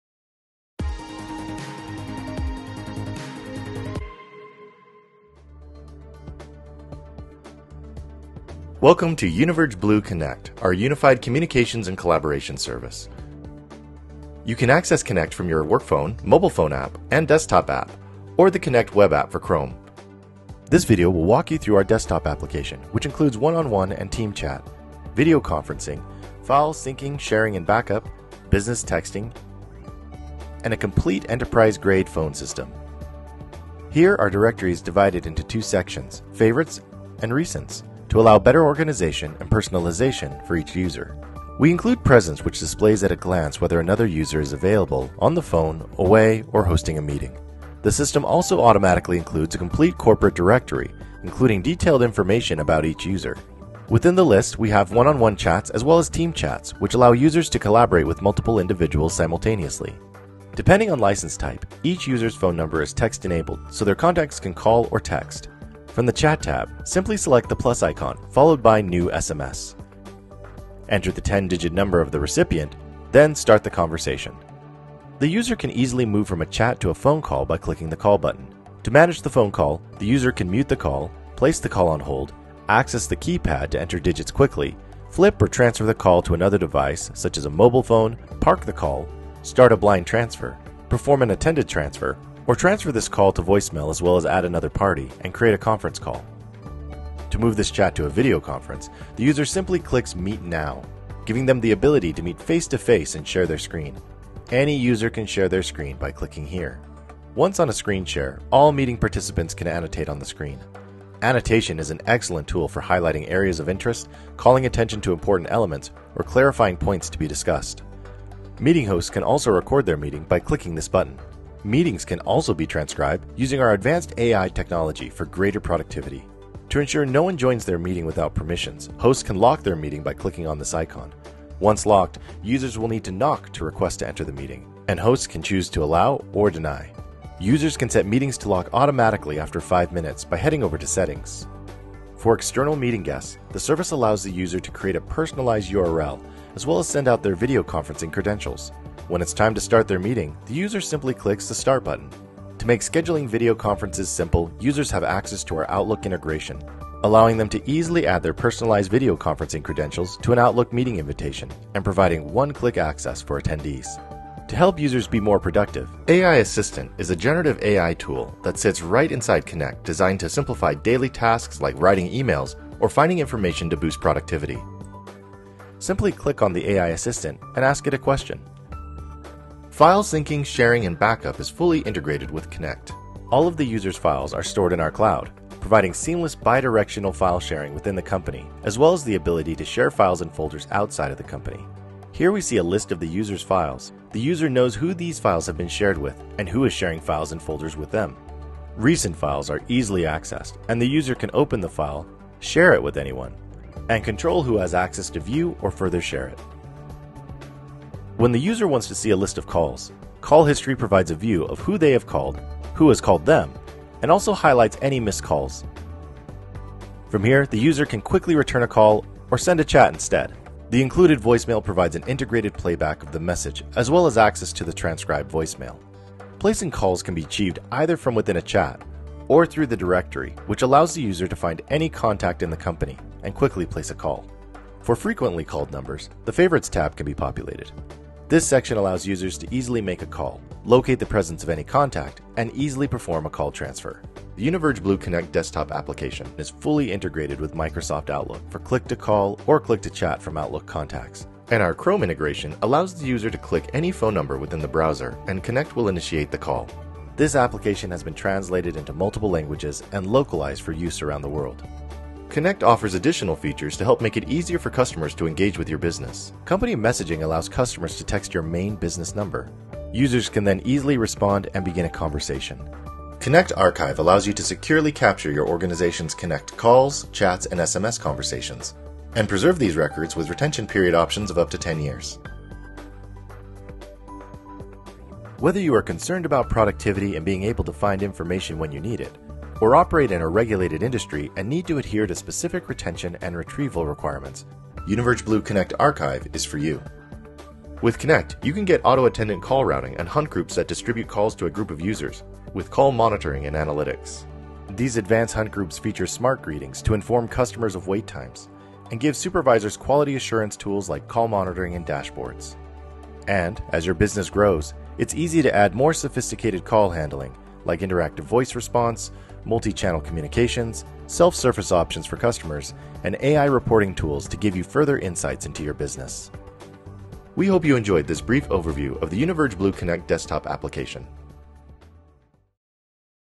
UNIVERGE BLUE CONNECT Desktop App
Discover the power of seamless collaboration with the UNIVERGE BLUE CONNECT desktop app! This all-in-one solution combines essential tools like phone systems, chat, SMS, video conferencing, file management, AI assistants, and archiving into one easy-to-use platform. Say goodbye to the hassle of switching between apps and increase your team's productivity and collaboration like never before!

Key Features of UNIVERGE BLUE CONNECT:

-Access your work virtually anywhere, anytime, on any device with ease.
-Enhance efficiency with AI-powered transcription of calls, meetings, and voicemails.
-Boost collaboration with integrated chat, text messaging, video conferencing, screen sharing, file sharing, and backup.
-Utilize the built-in Generative AI tool for quick information access during chat sessions.
-Extend video conferencing to your conference rooms for seamless connections between in-office and remote workers.
-Get started in no time using your current phone number and preconfigured phones—no complex hardware required!
-Transform your mobile device into a part of your CONNECT phone system for unmatched flexibility.

UNIVERGE BLUE CONNECT offers 100+ enterprise-grade features with superior network call quality and uptime, ensuring your business stays ahead. Manage your system and access call reports from a single web-based portal for streamlined operations.

Learn how UNIVERGE BLUE CONNECT can revolutionize your workplace communication and drive your business forward!